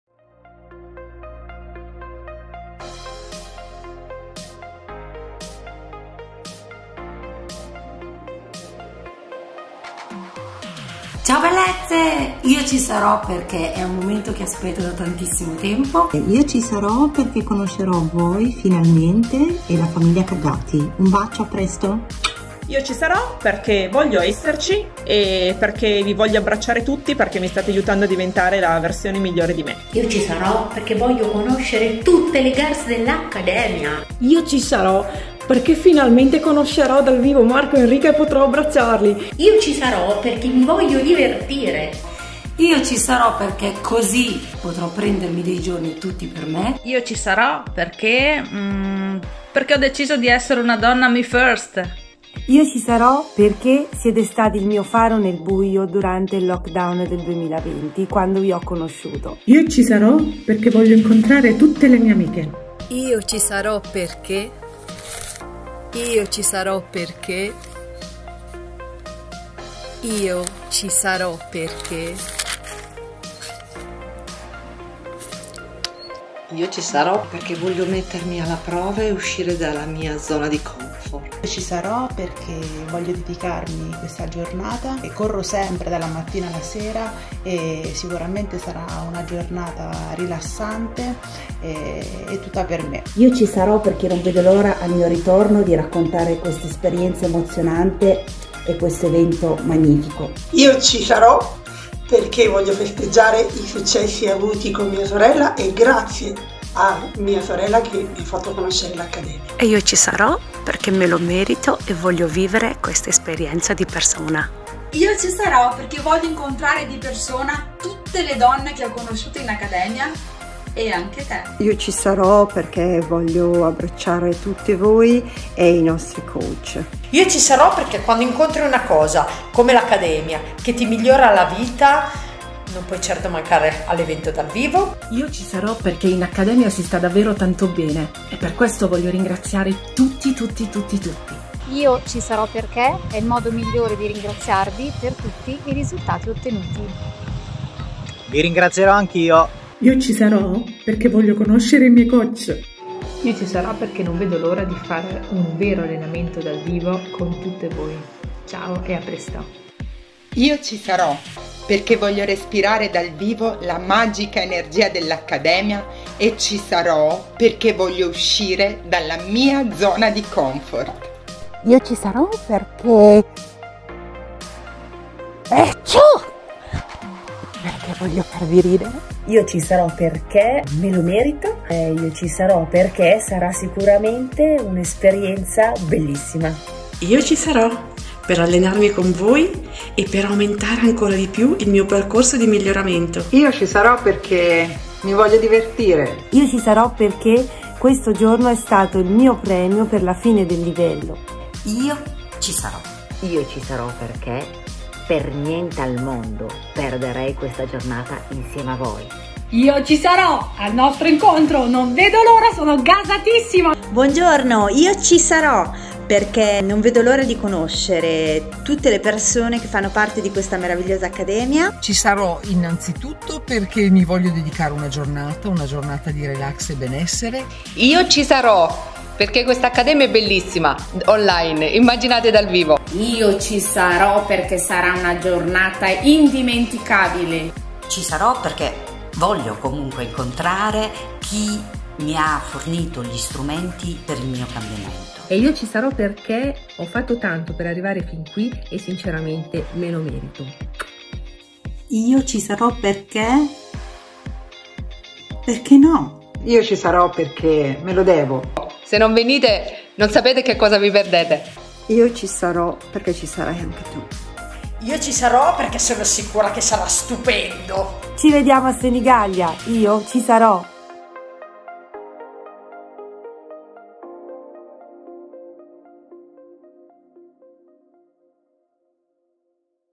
Primo Evento dal VIVO Bella e in forma per sempre: loro ci saranno e tu?
In questo video alcune delle centinaia di ragazze che parteciperanno al PRIMO evento dal VIVO di Bella e in forma per sempre, ti fanno "assaggiare" l'energia e la voglia di godersi una giornata straordinaria il 21 di Maggio 2022 a Senigallia! 🤗